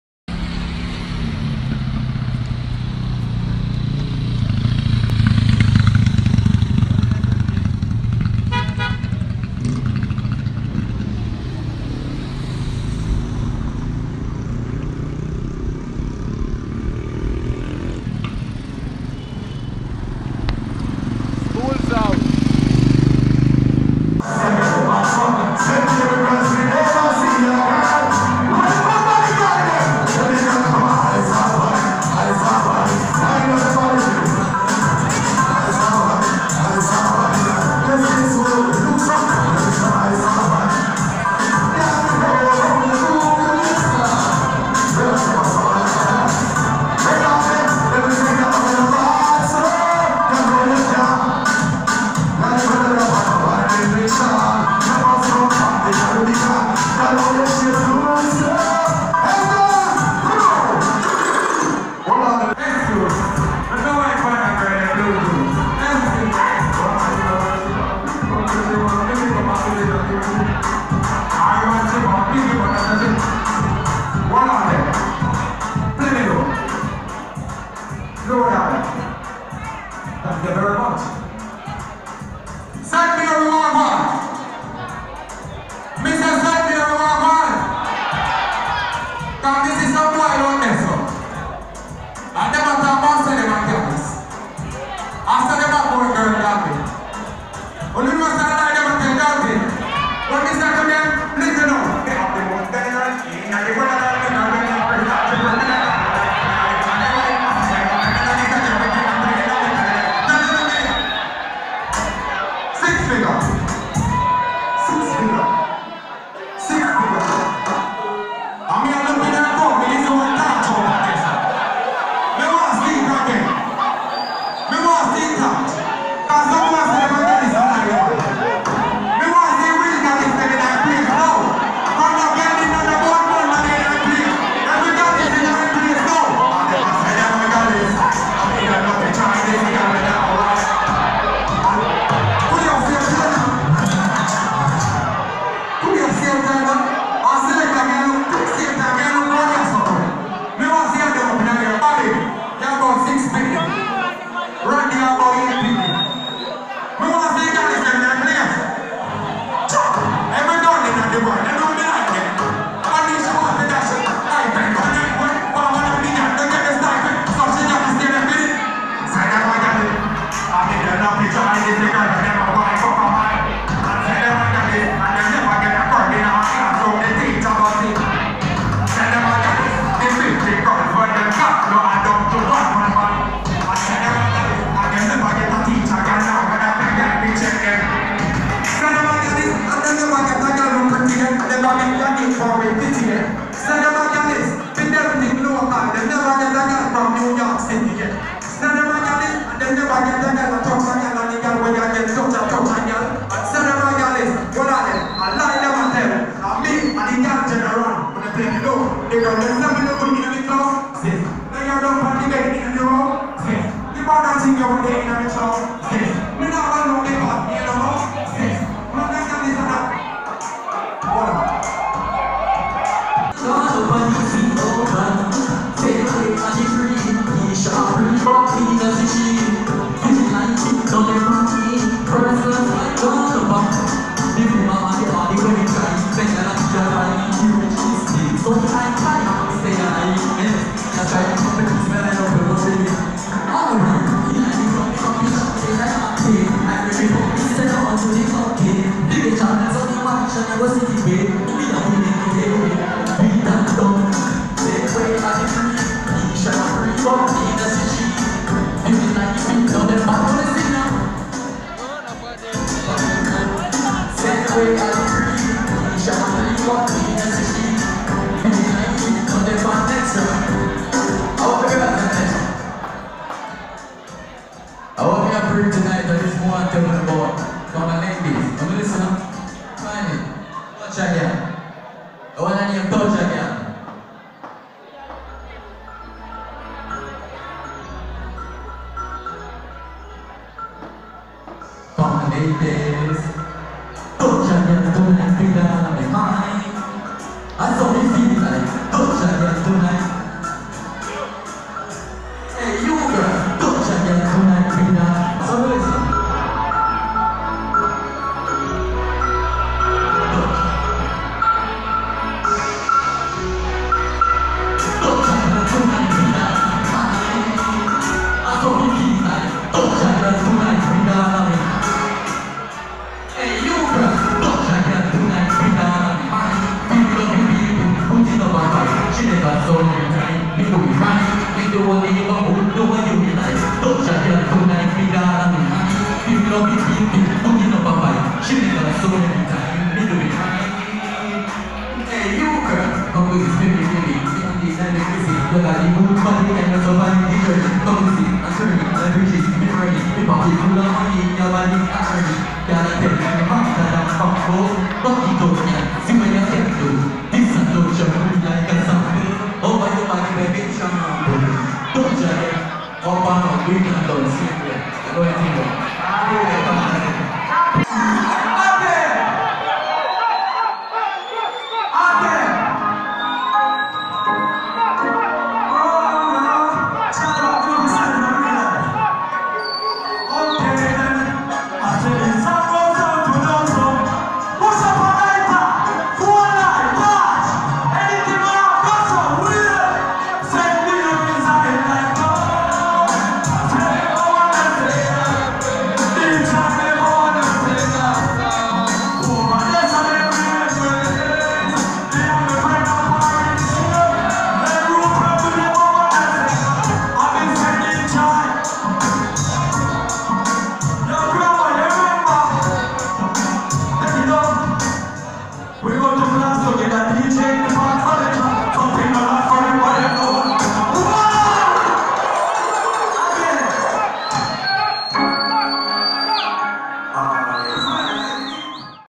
St Mary High School (Skoolz Out) 2018
Charly black, Devin Di Dakta, Authentic Luke, Deep Jahi Performance At The St Mary High School (Skoolz Out) 2018